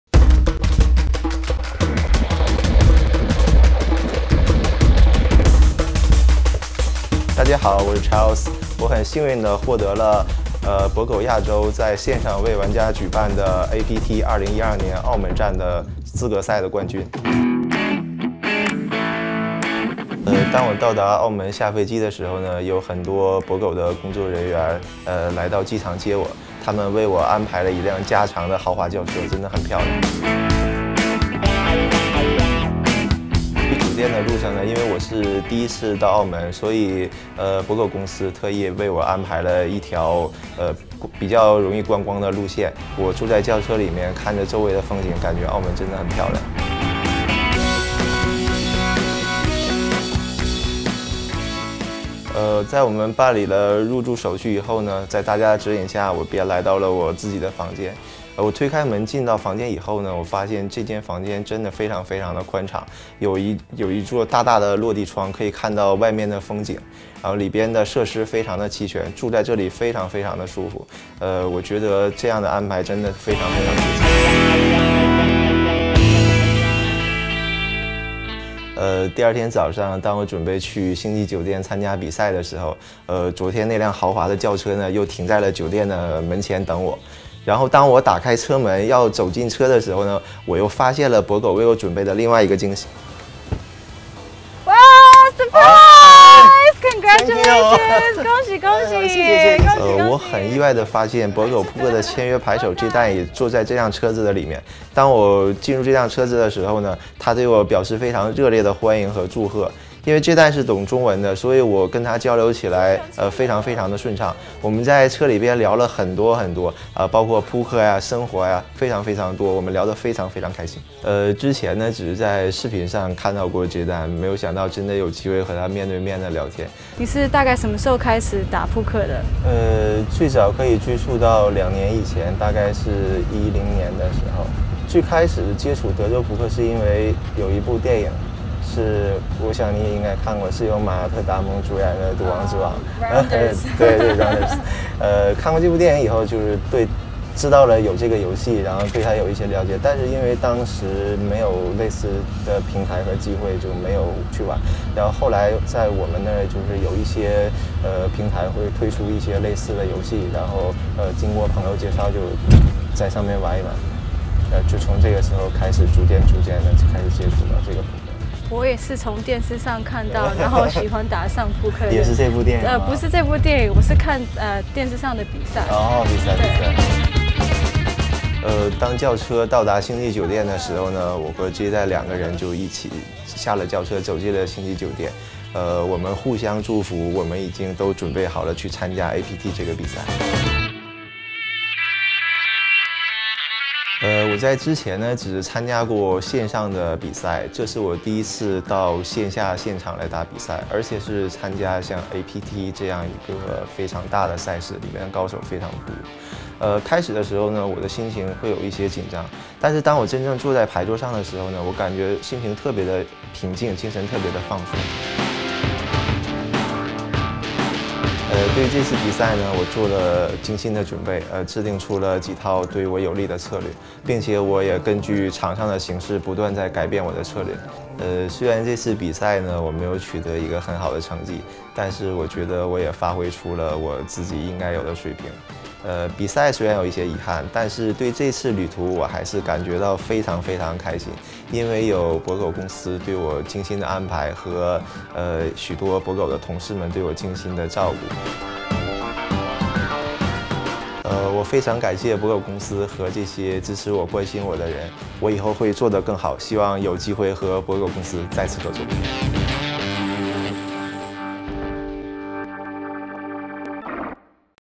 Bodog88 APT Macau 2012 Winner Interview (Thai subtitles)
Charles Dunn is given VIP treatment after winning Bodog88's Win a Trip to Macau promo. Charles won a $6,500 prize package which included a $3,300 for the Main Event buy-in, an 8 night stay in the Galaxy Macau Hotel, $1,520 spending money and a Bodog88 swag bag. Visit Bodog88 for a chance to win more promos